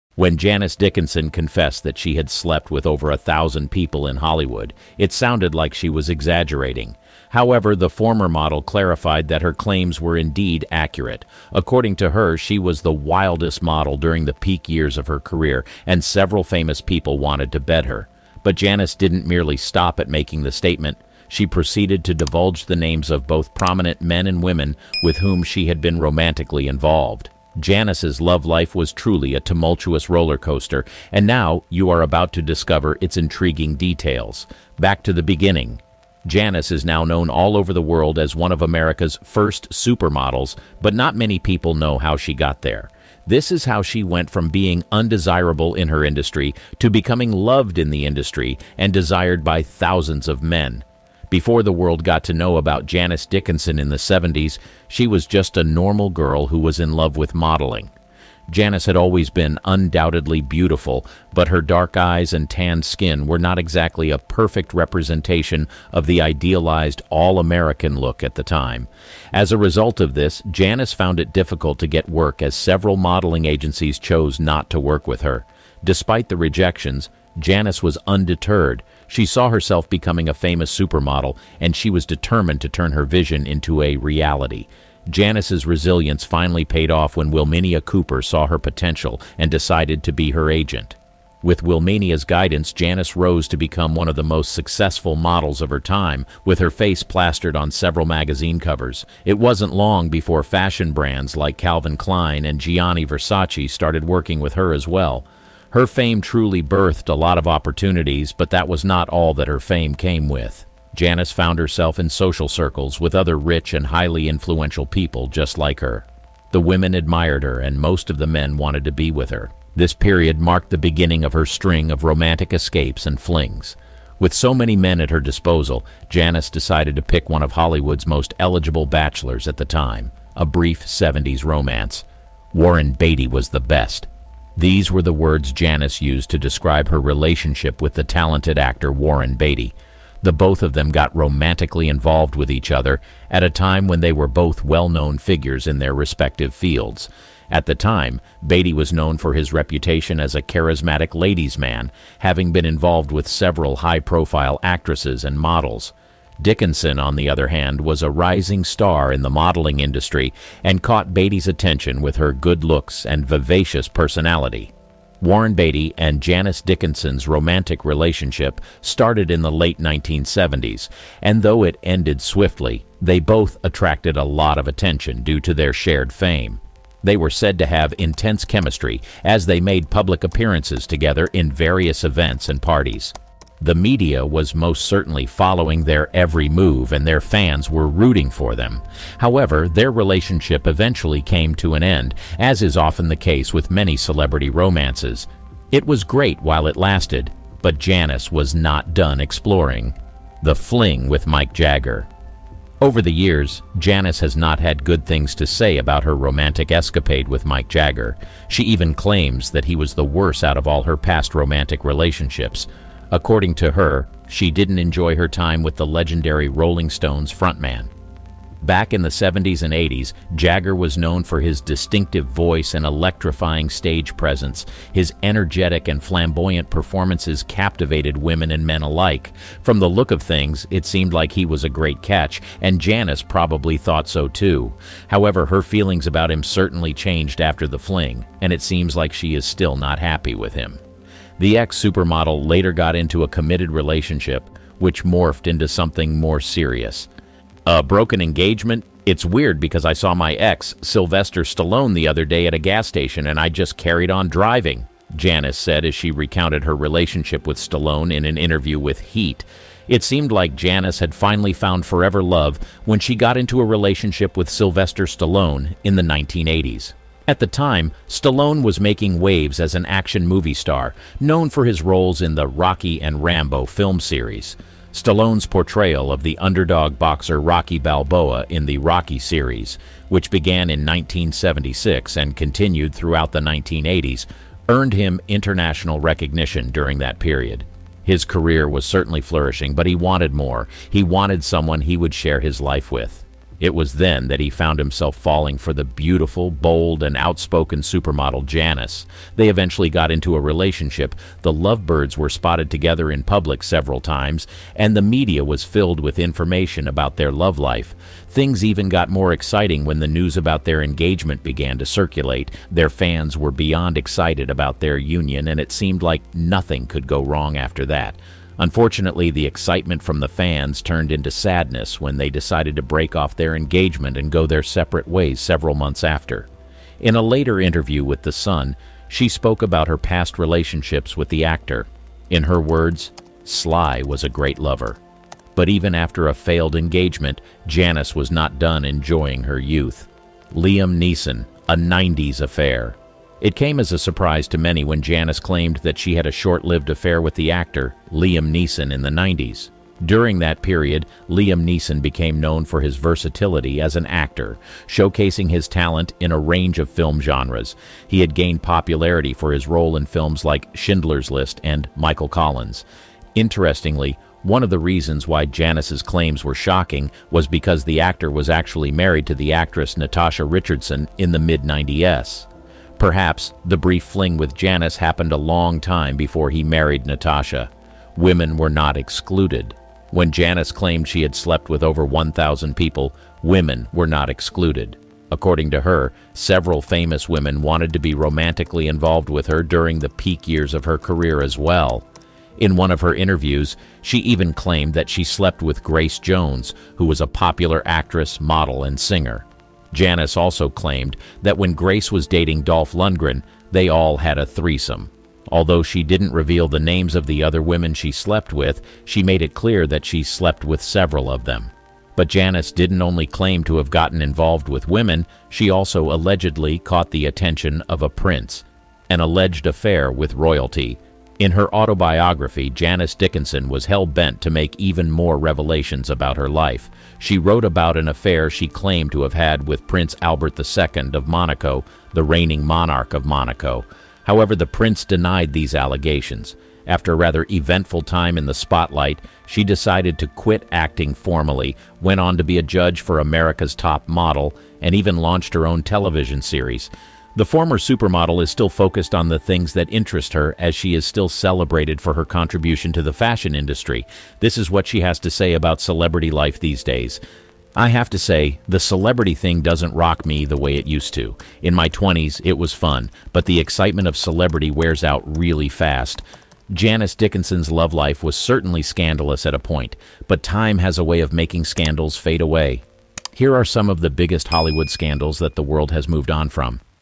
Not Only Did Janice Dickinson Sleep with Everyone, But WOMEN?
Janice Dickinson was known for her wild lifestyle and eclectic taste in partners, but did you know that she had relationships with women too? In this shocking video, you'll discover the hidden side of Janice Dickinson's life. From secret trysts to unexpected romances - let your curiosity and fear be your guide as you uncover a never-before-seen side of Janice Dickinson!